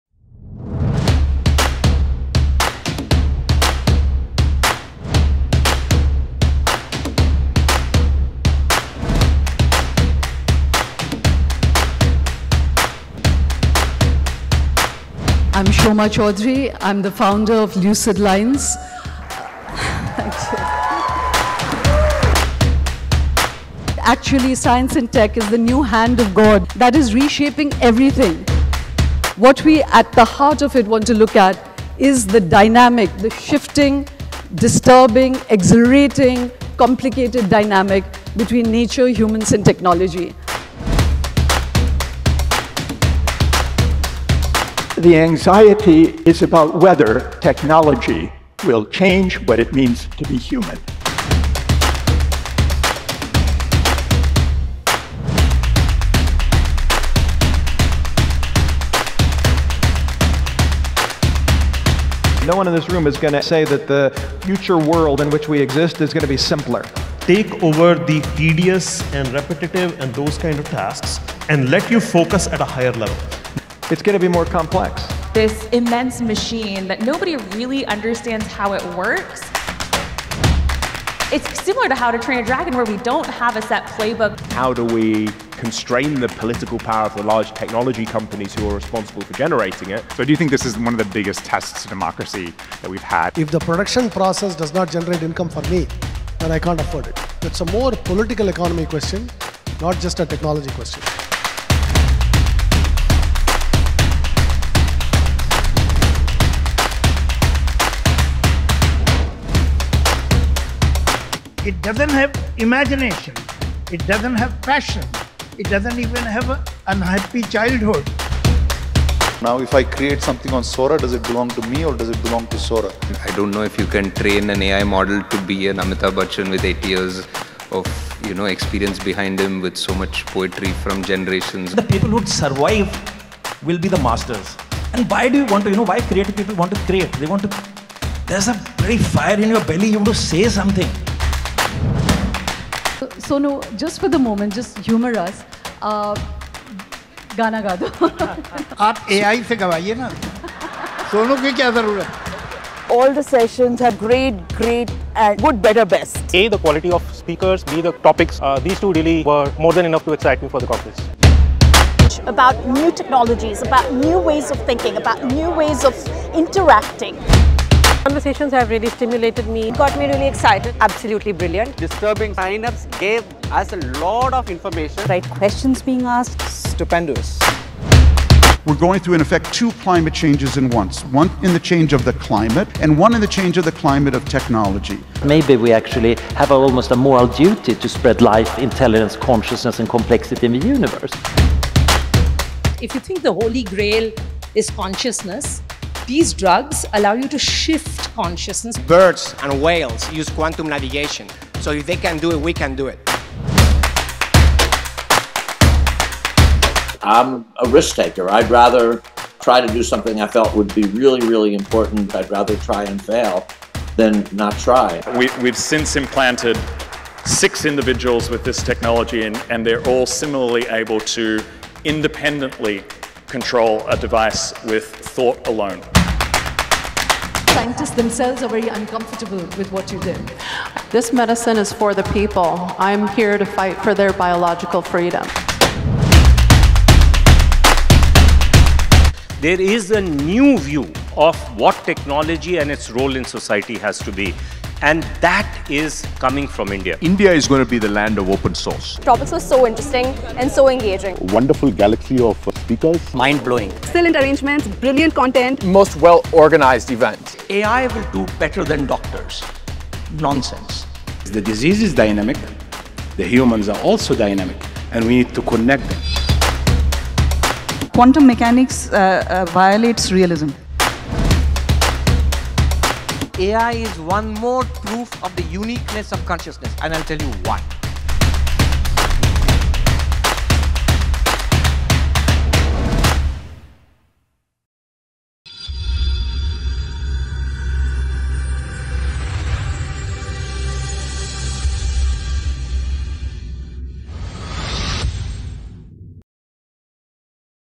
SYNAPSE 2024 | SNAPSHOTS
We are delighted to share snapshots of SYNAPSE, the world’s most thought provoking science tech society conference.

Our inaugural edition in 2024 brought together 40 pathbreaking speakers. Technologists, scientists, entrepreneurs. Artists, economists, philosophers. Political leaders, national security experts, ethicists.

And 1300+ curious, engaged, and influential audience members.

To discuss science and tech, the new 'hand of god' in our lives, reshaping everything. To witness the breakthroughs – AI to gene editing. Age reversal to quantum. And to debate its power, peril, and promise. From geopolitics to jobs, cosmos to consciousness to creativity, startups to space.

Incisive. Insightful. Visually stimulating. A mind-expanding weekend.